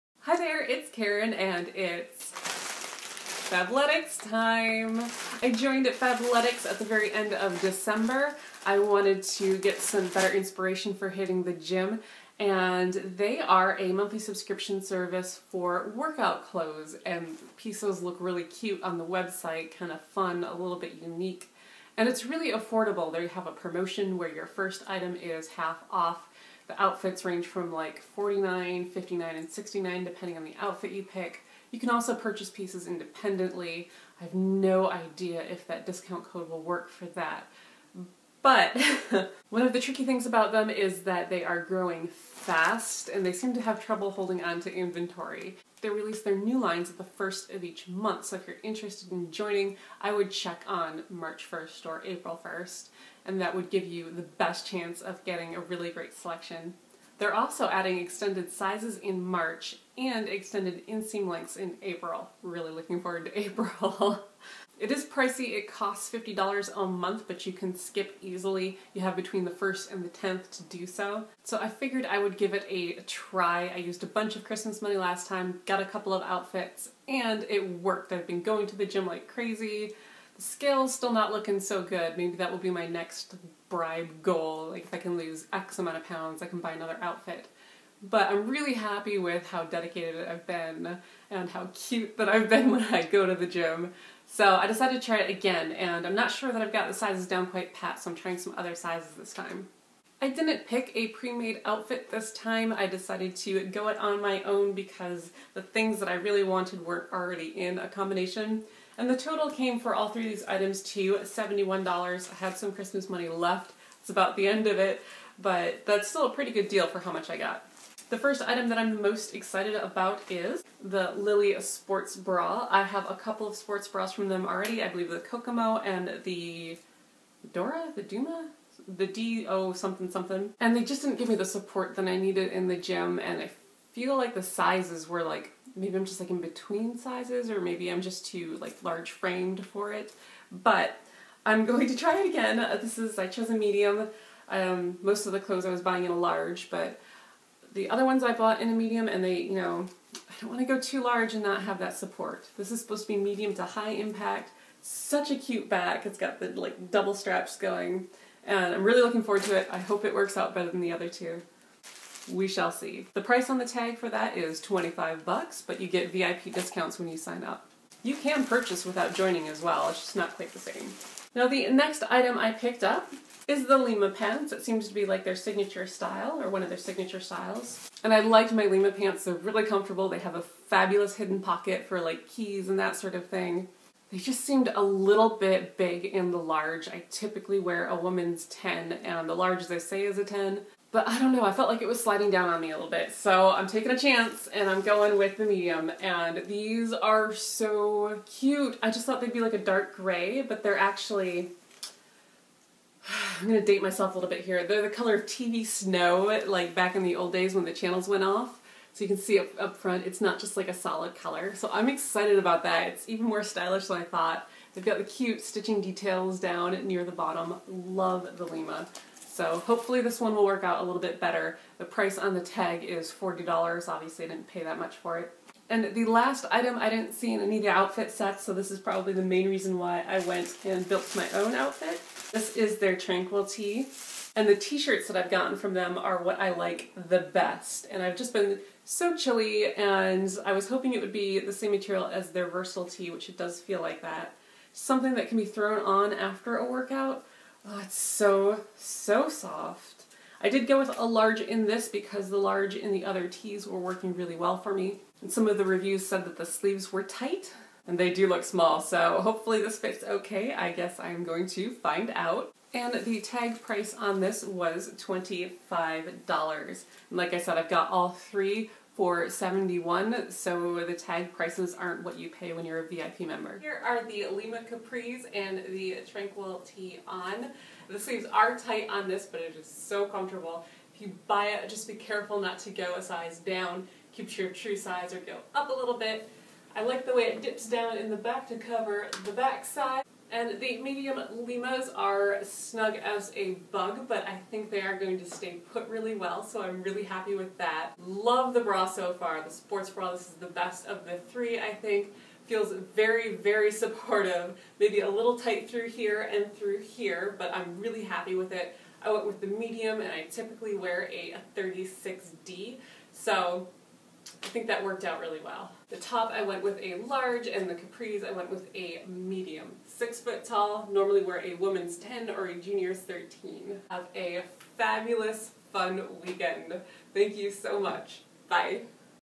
Fabletics February 2014
This is a quick review/first impression of my February Fabletics picks.  Fabletics is a newer subscription service for work out cloths.  It's $50/mo but easily skipable.  You can also purchase without joining, but it is no where near as great a value.  New outfits are released on the 1st of each month and start at $50 for two items.  They currently have a promotion that will snag you half off your first outfit, which is a crazy steal.  High fashion, good value, but mid range quality.  If you want to learn more you can check them out at:
(just your average everyday link)